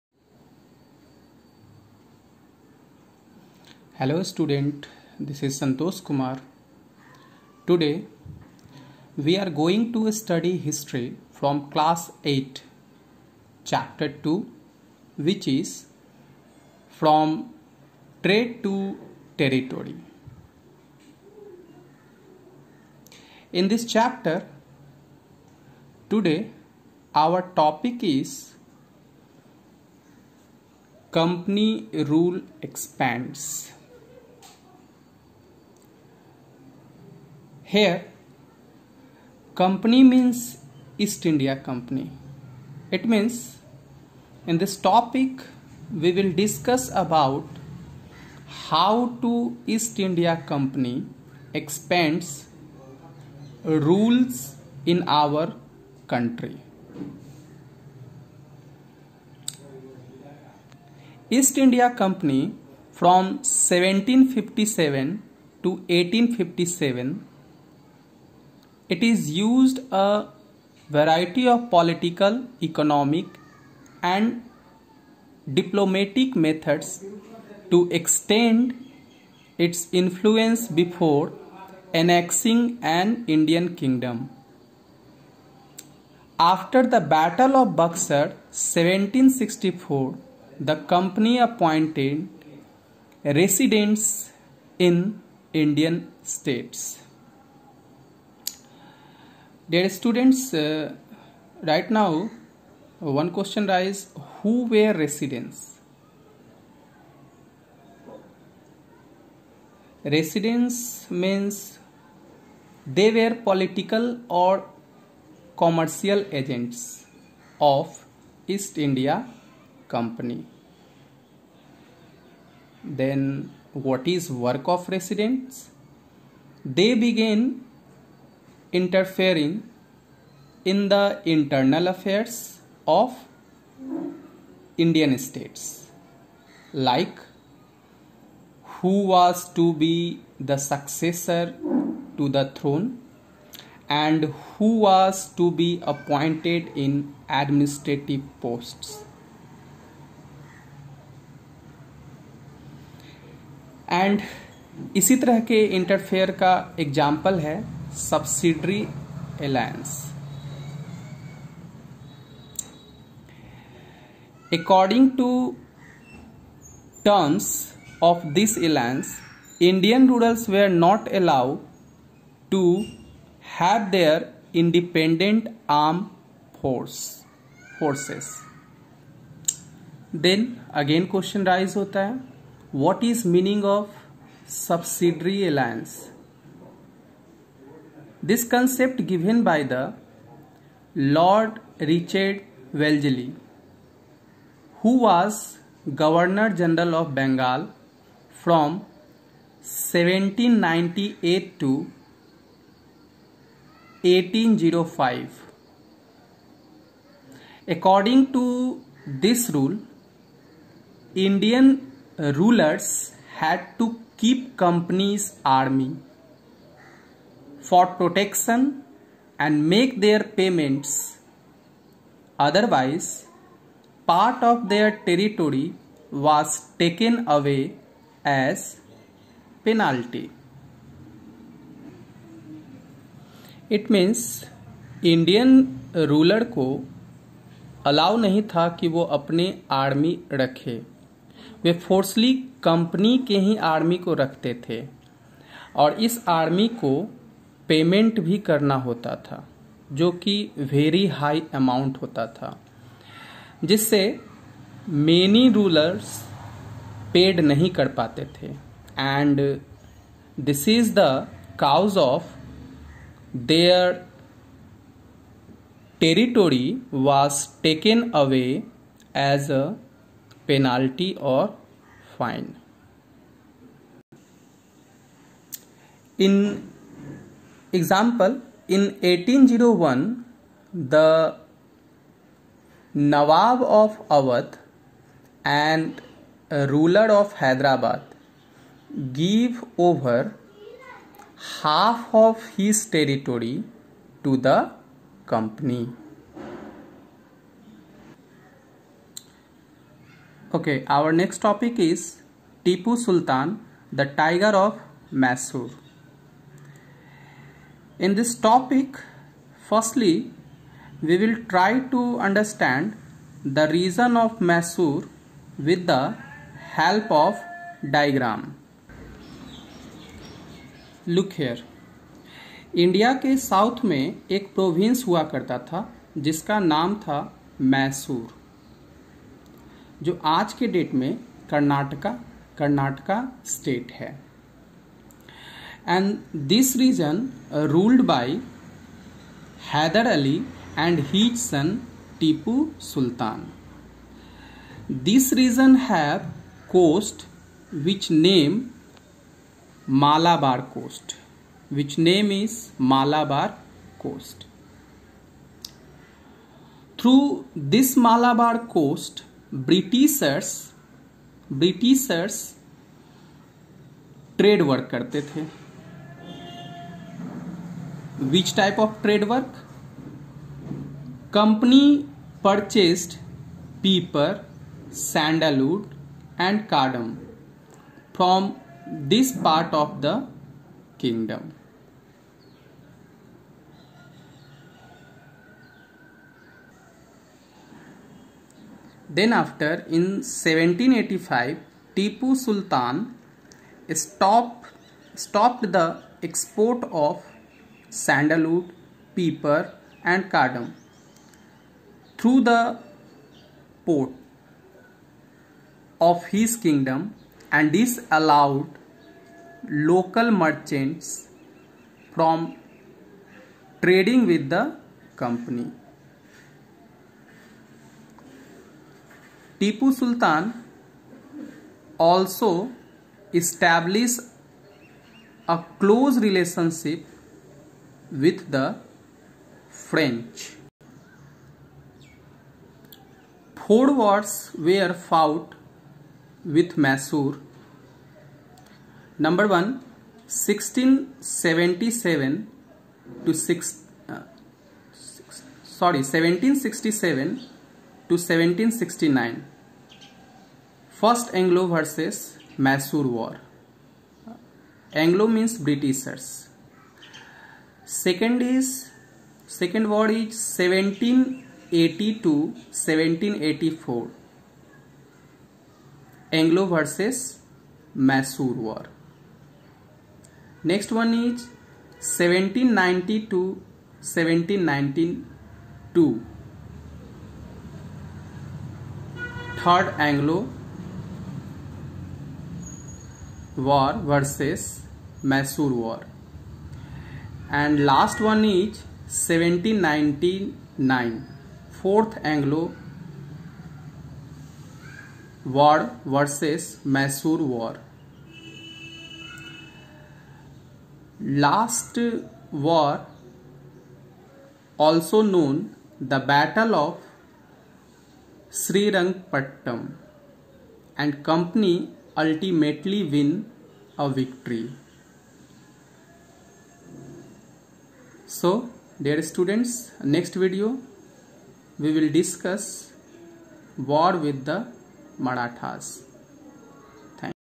Santosh kumar Class - 8 Subject - History Chapter - 2 (From Trade to Territory) Part - 4
Santosh kumar
Class -  8
Subject - History
Chapter - 2 (From Trade to Territory)
Part - 4